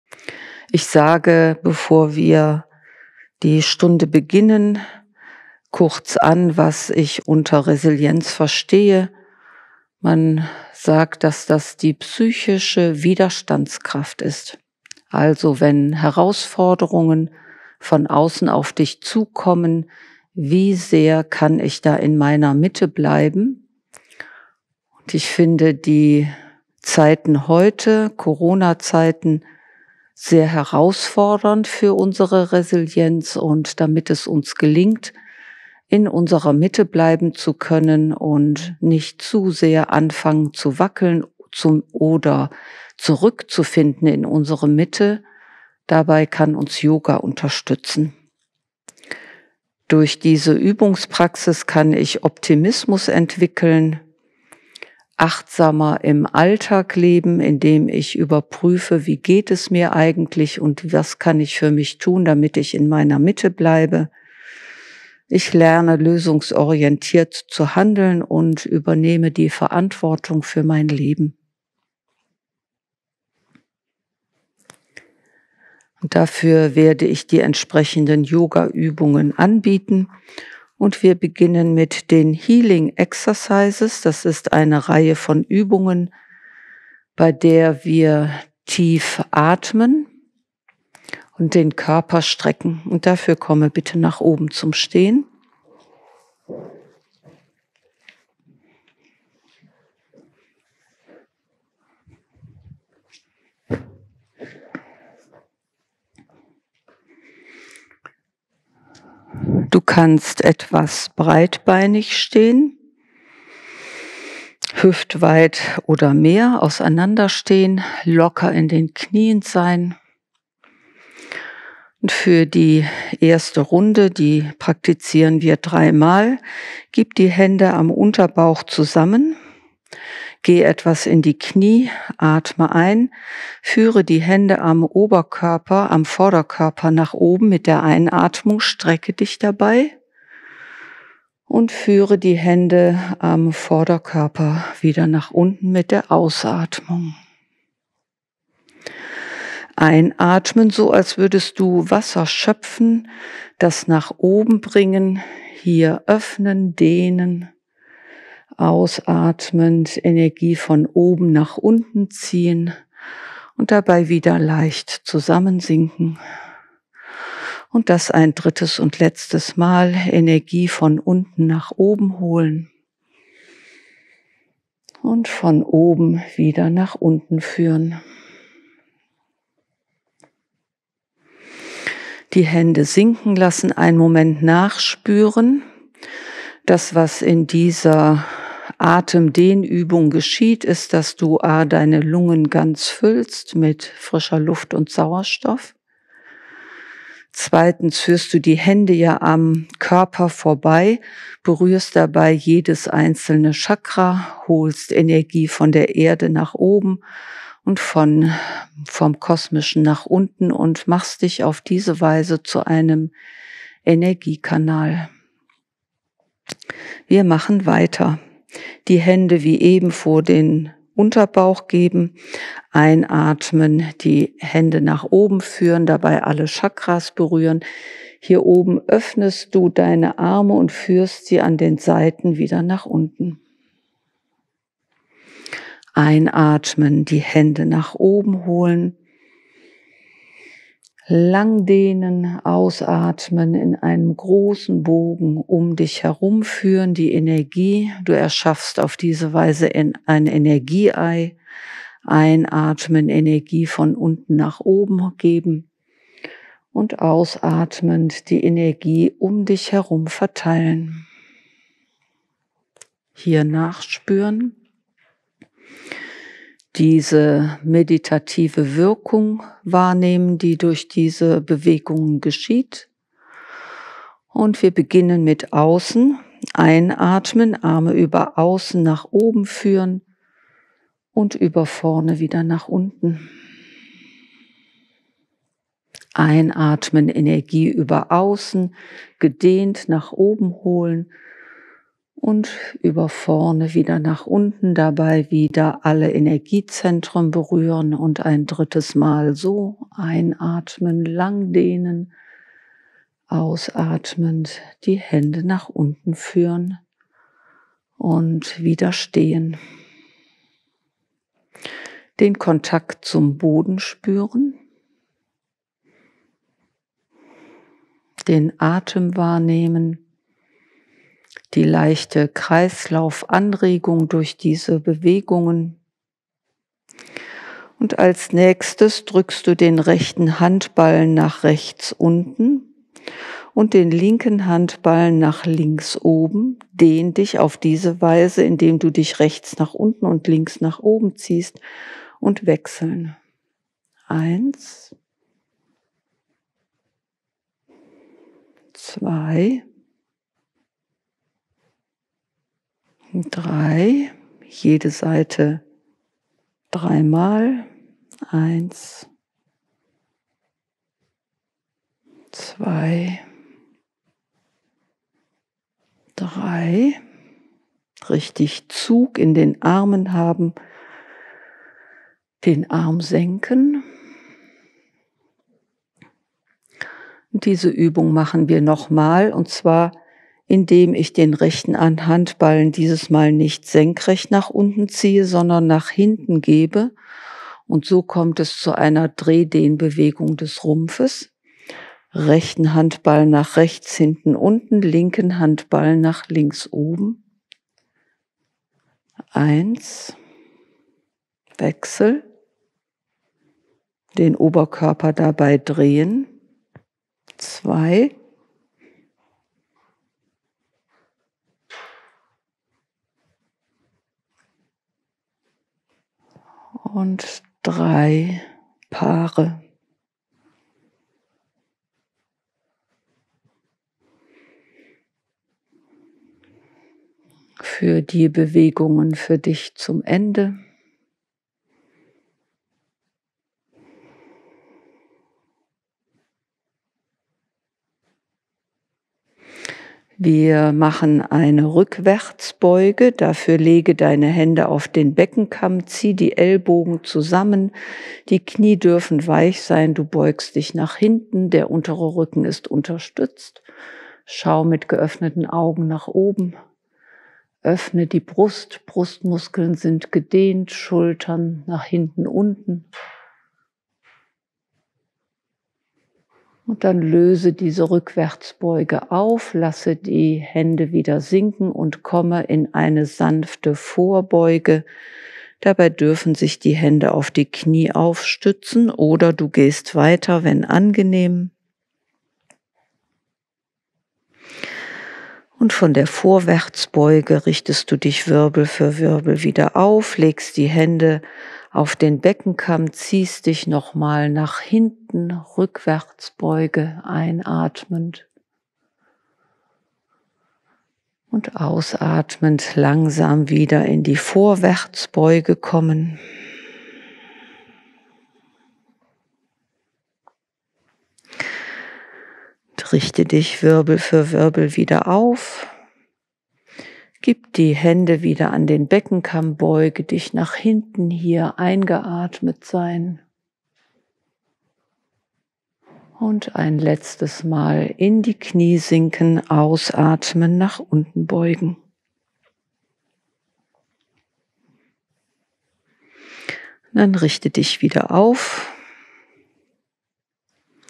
Überstehe jede Lebenssituation mit YOGA// Stärkung der Resilienz// Yogatherapie// Yoga Vidya Ashram
Stärke deine psychische Widerstandsfähigkeit mit diesen Übungen aus der Yogatherapie. Diese "Healing Exercises" machen dich widerstandsfähiger gegenüber herausfordernden Lebenssituationen. Probiere es aus und gib uns gern Feedback!

Resilienz ist eine besondere Kraft der Psyche, Belastungen auszuhalten – eine ausgeprägt lebensmutige Haltung. Ein resilienter Mensch lässt sich von Schicksalsschlägen nicht aus der Bahn werfen, sondern kommt rasch wieder auf die Beine und bewältigt sein Leben wie zuvor. Das ist jene Lesart, die man in Ratgebern findet oder auf Wochenendseminaren zur besseren Bewältigung von Krisen hört.